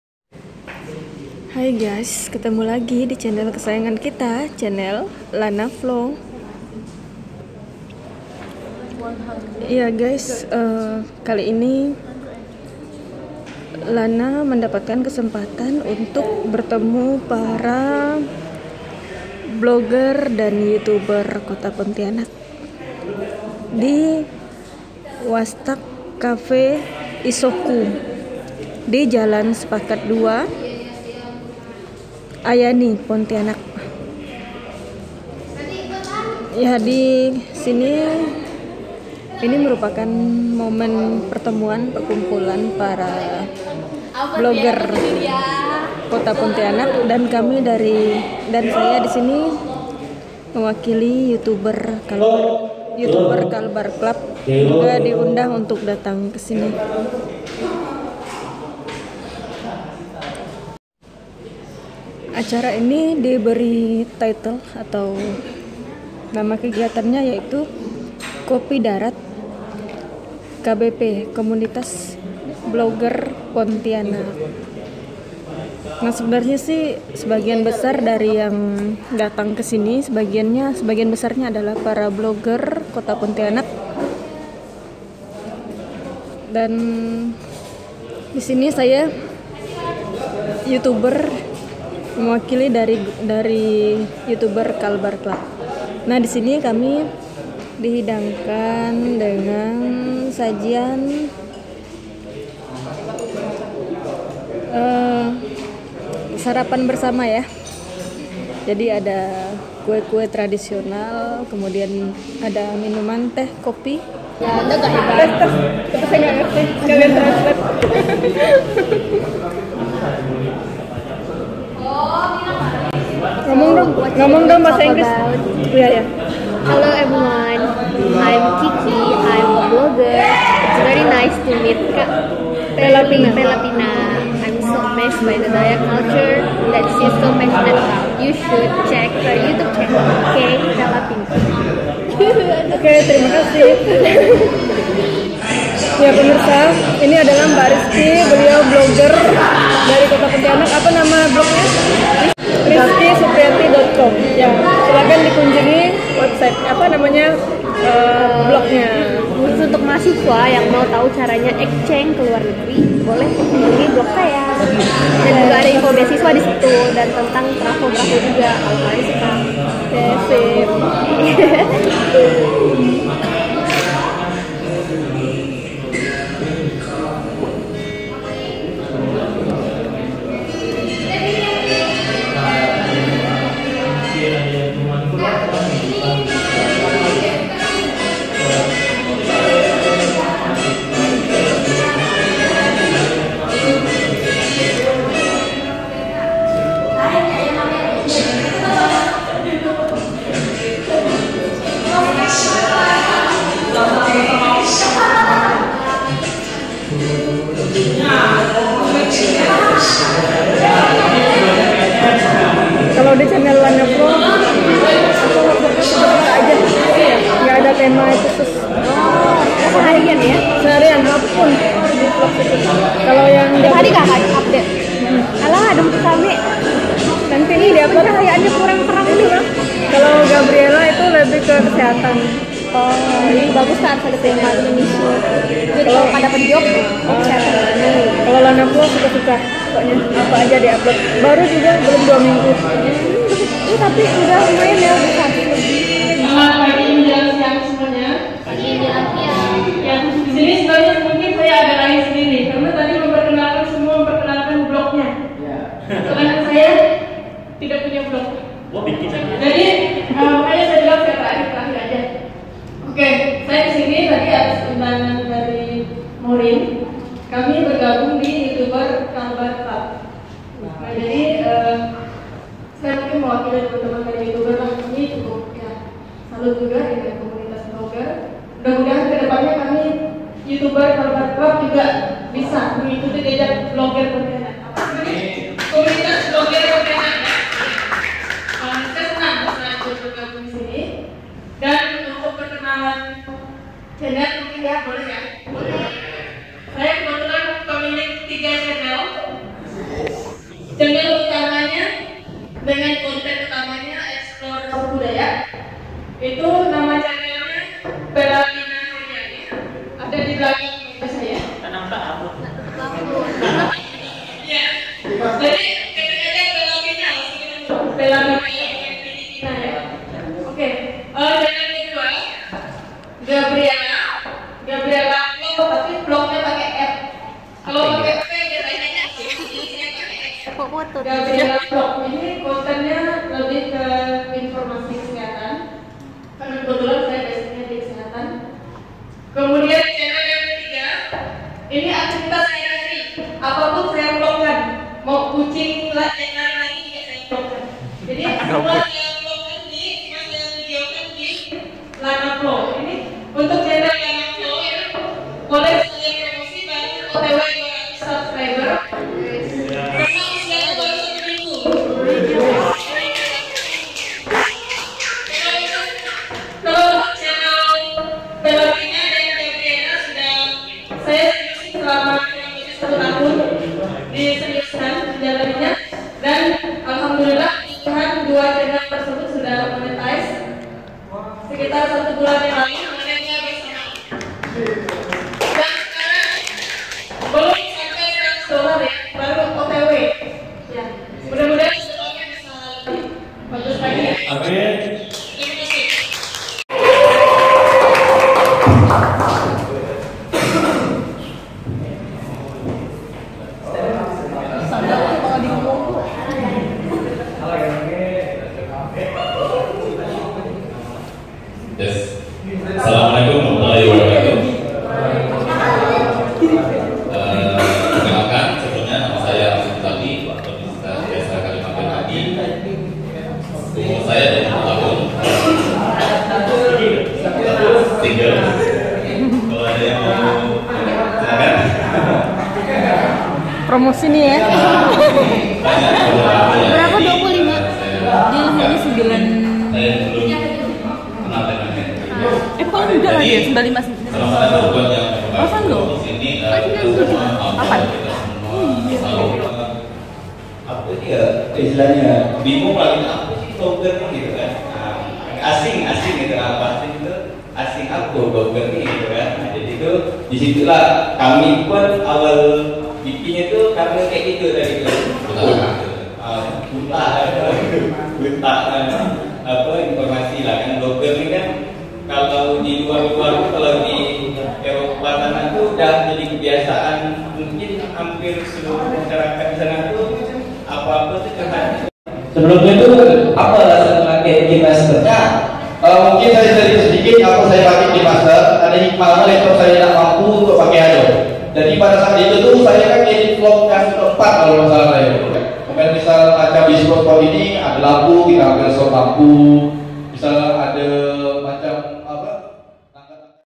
Youtuber ketemu Blogger, Kopi darat Komunitas Blogger Pontianak
Hai semua. kali ini Lana mendapat kesempatan bertemu para Blogger Kalimantan Barat dalam acara Kopi darat Komunitas Blogger Pontianak